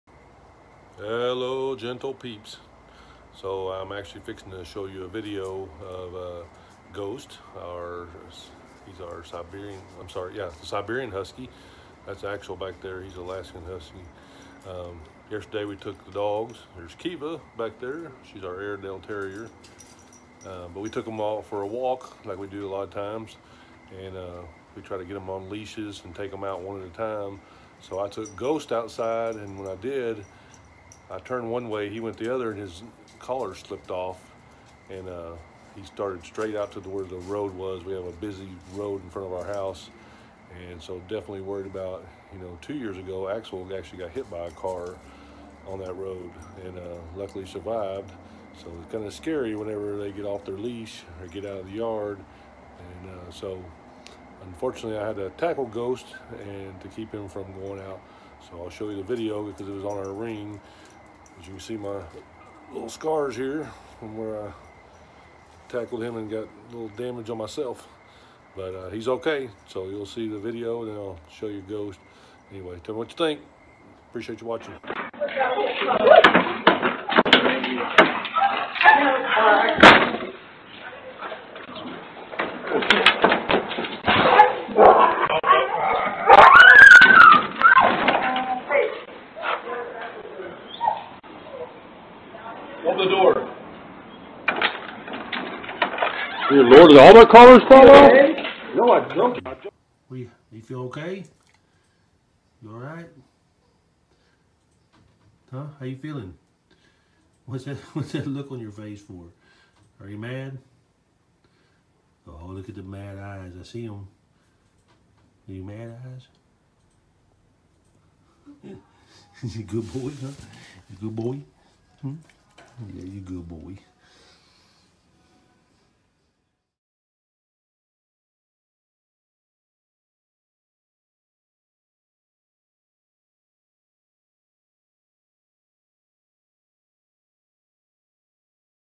Tackling Husky to save him from running into the road 🦮
Ghost our two year old Siberian Husky, as well as Axl Rose our Alaskan Husky, and Kiva our Airedale Terrier, love to go for walks. We have to use leashes. Two years ago Axl got out and was hit by a vehicle. He was hurt bad but lived and fortuntely has healed well and is all good. But we live off a busy main road here in town. And Ghost has almost been hit also. I have fixed all fence issues so they  never get out now. But today as I took Ghost out the front door, as I turned his collar was too loose and it slipped off. Scared me to death, all I could see was him running to the road and getting hit, so I had to tackle him unfortunetly to save him from running away. Ghost is fine, and I had some good scrapes. Our front door Ring caught the action. :)

Hi, I am Jack Lacy, welcome to my video and my YouTube.